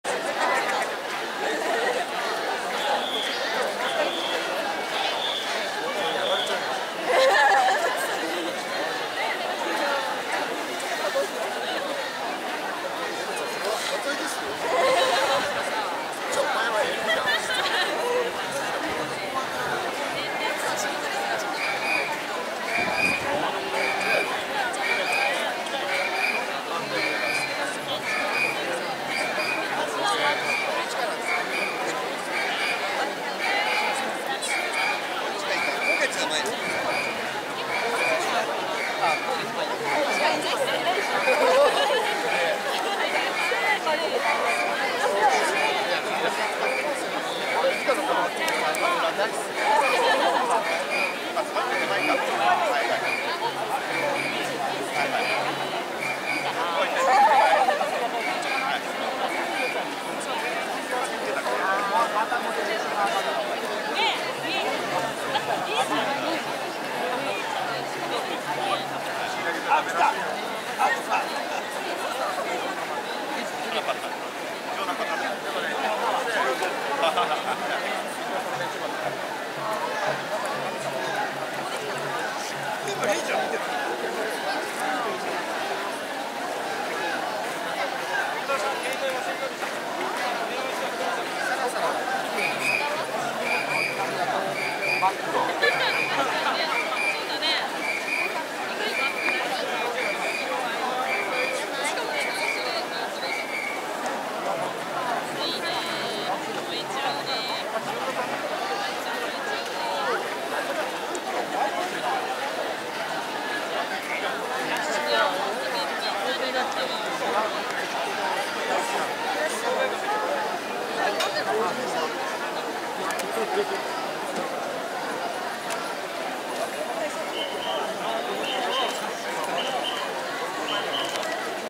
三社祭 町内神輿 本堂裏 発進 4-4
17-05-20 浅草神社 三社祭
浅草寺本堂裏広場に参集した神輿が、一基ずつ発進。
浅草氏子44ヶ町の町内神輿約100基の渡御。